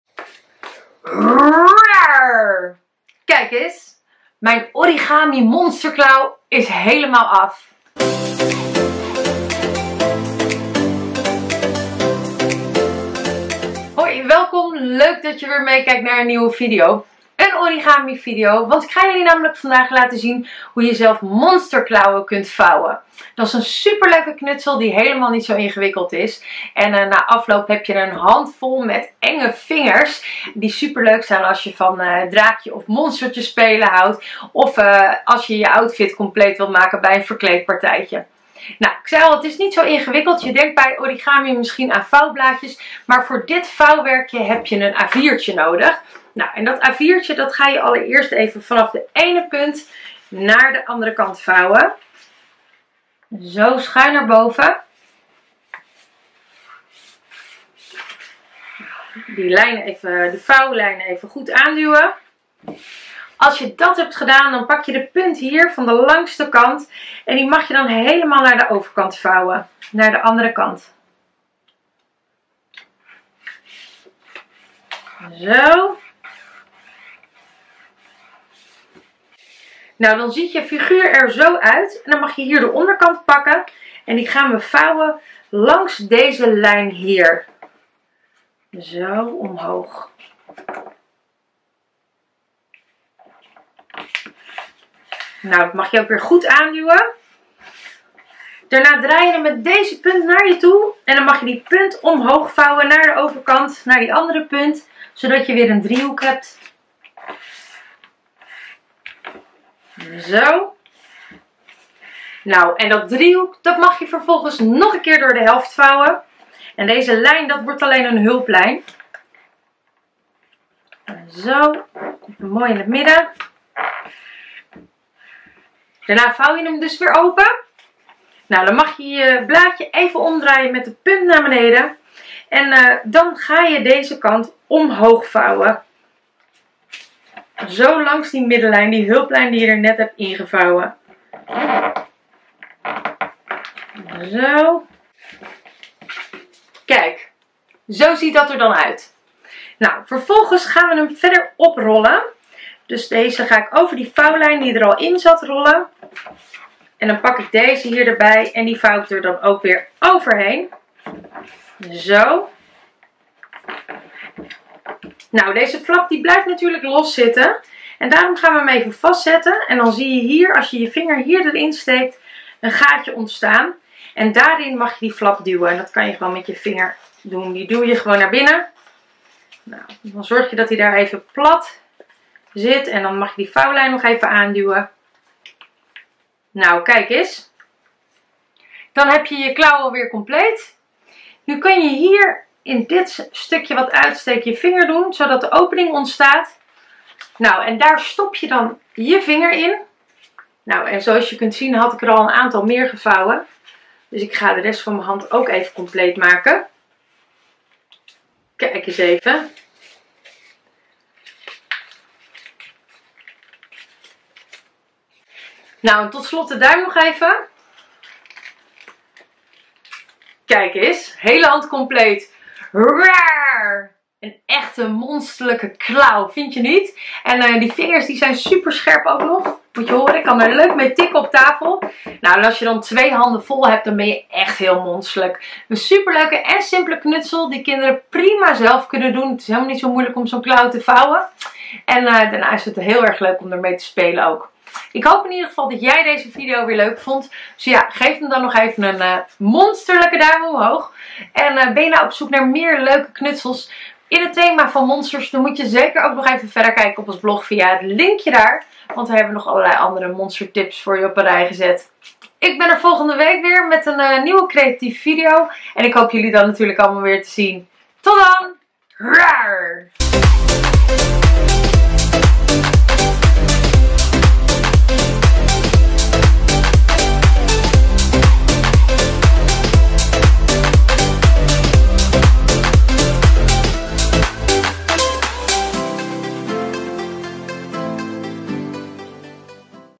Monster klauwen vouwen - Zo maak je stap voor stap origami klauwen!
Met deze papieren origami klauwen om je vingers kun je iedereen de stuipen op het lijf jagen. Ze zien er mega indrukwekkend en gevaarlijk uit. Een super leuk knutselidee voor liefhebbers van draken, dino’s en monsters. En het is heel makkelijk om de klauwen te vouwen. In deze video kun je stap voor stap zien hoe het moet.